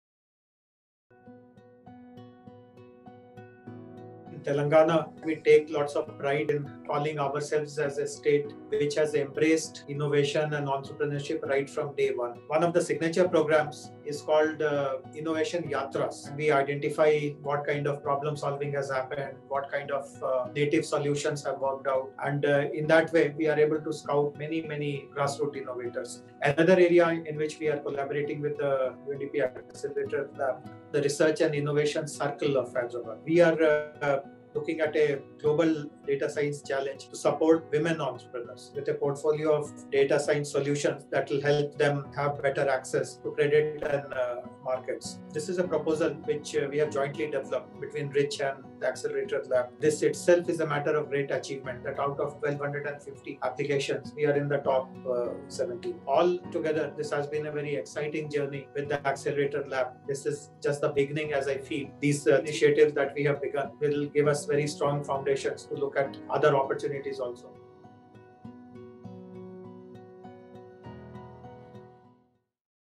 AccLabTurns1: We identify problem solving & native solutions.
“In our collaboration with AccLabIndia, We identify what kind of problem solving has happened, what kind of native solutions have worked out so that we are able to scout many grass root innovators”

Jayesh Ranjan, Principal Secretary, Department of  Industries & Commerce (I&C) and Information Technology (IT), Government of Telangana talks about the partnerhsip with AccLabIndia as it turns one.